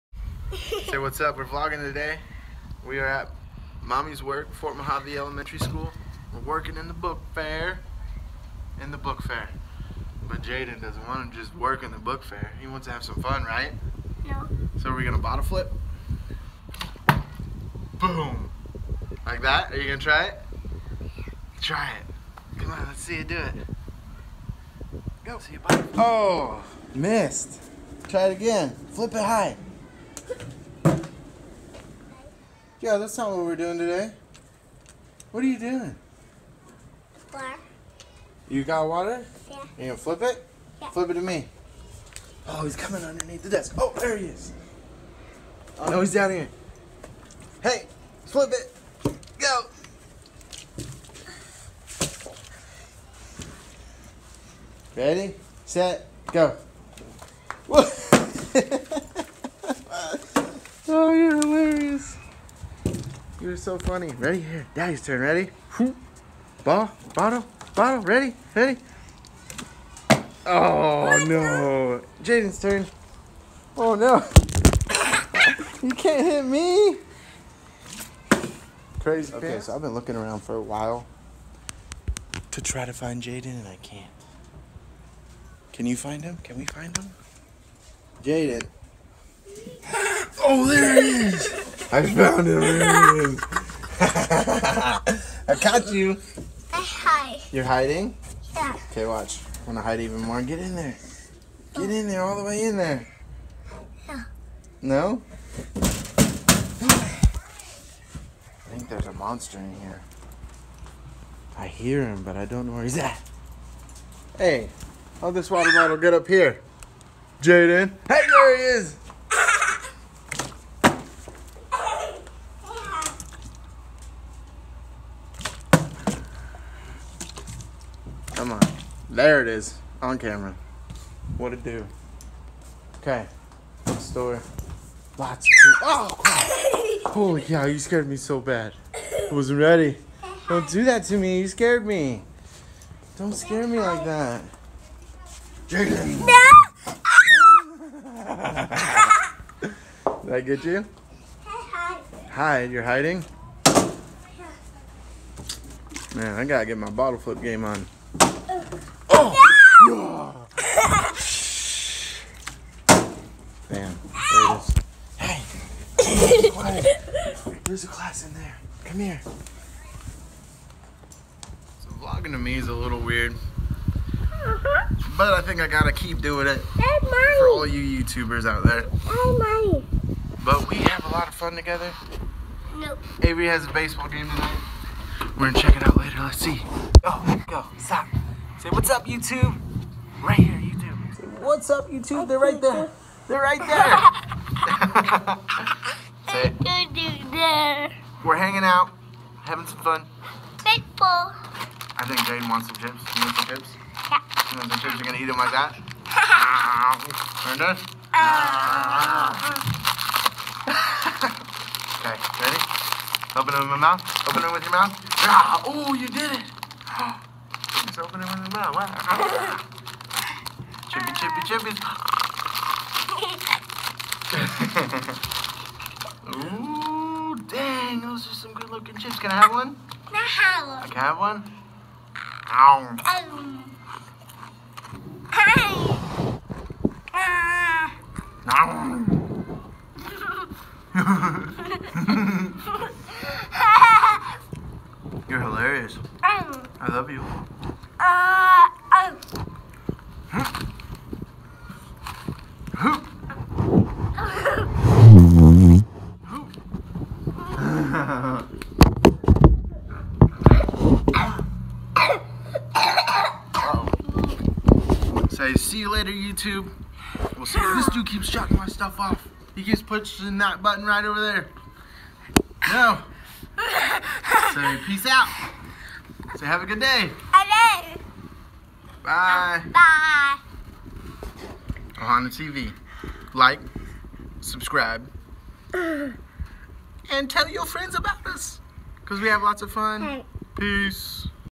Fun Times At FMES!!!
My boy OhanaTeeVJai and I where at mommy's school working the book fair and having a little fun!! It's the first of many Vlog's!! Let your friends know and there are more videos to come.

Vlogs are not my thing right now but I am trying hope you all like it!!!